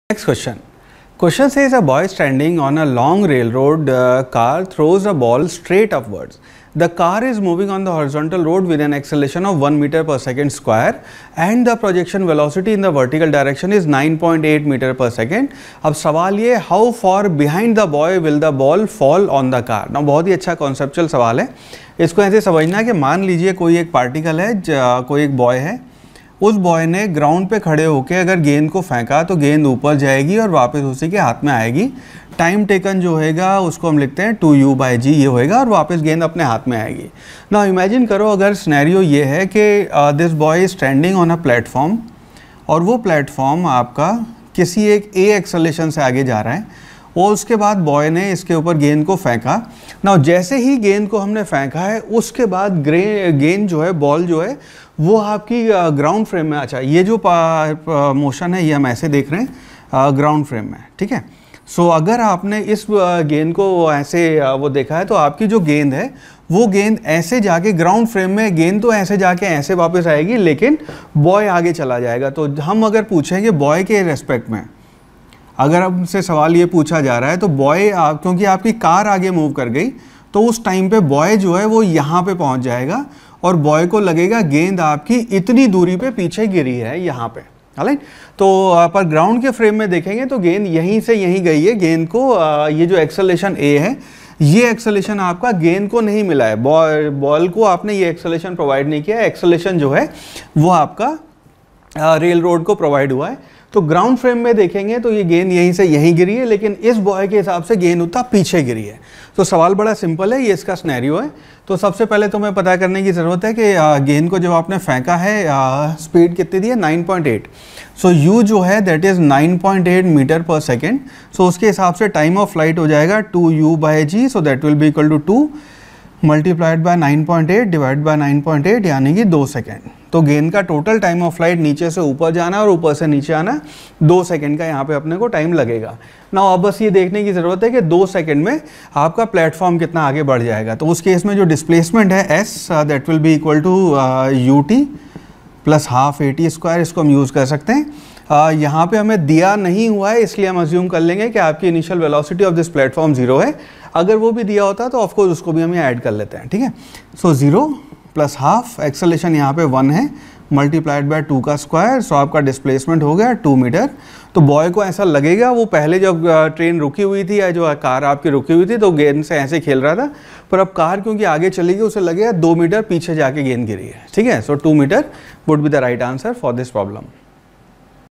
HCV: A boy standing on a long railroad car throws a ball straight upwards. The car is moving on the
A boy standing on a long railroad car throws a ball straight upwards. The car is moving on the horizontal road with an acceleration of 1 m/s 2 and the projection velocity in the vertical direction is 9.8 m/s. How far behind the boy will the ball fall on the car ?

Solutions to HCV Class XI Rest & Motion Kinematics | JEE  NEET Physics | Doubt Counter